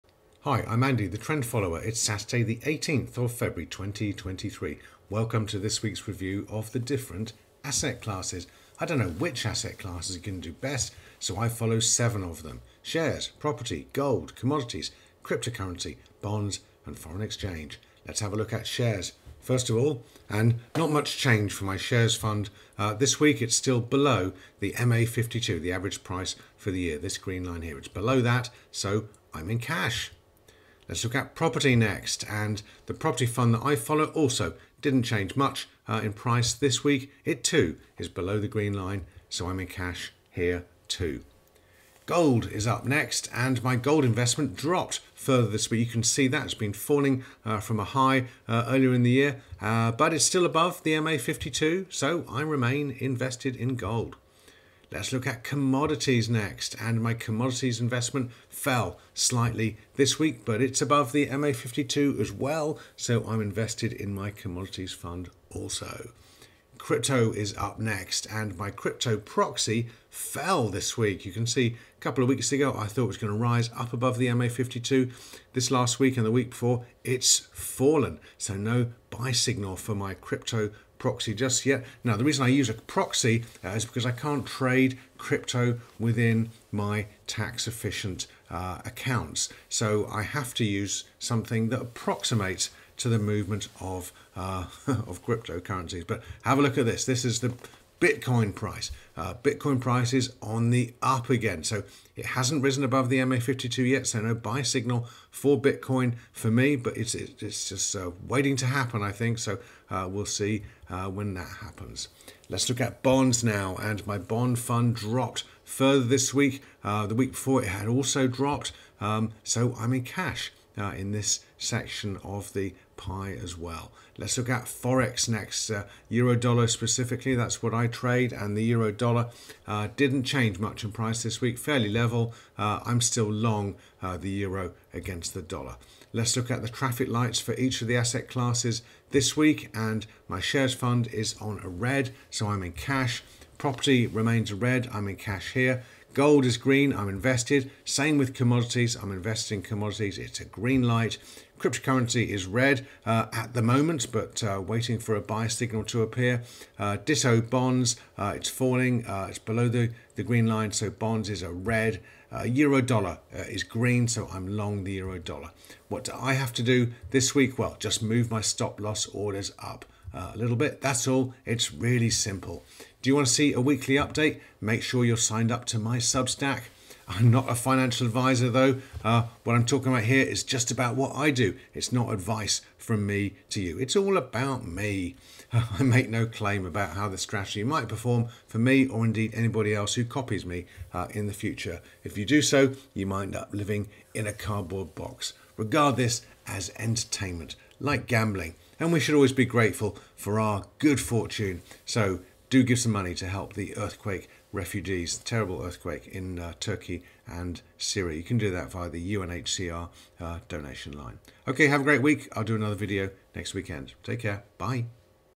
2023 02 18 weekly asset class review
My weekly review of the different asset classes. Do I want to be invested in: stocks, shares, property, gold, commodities, bonds, crypto, forex, or should I be in cash? Find out in this weeks short video!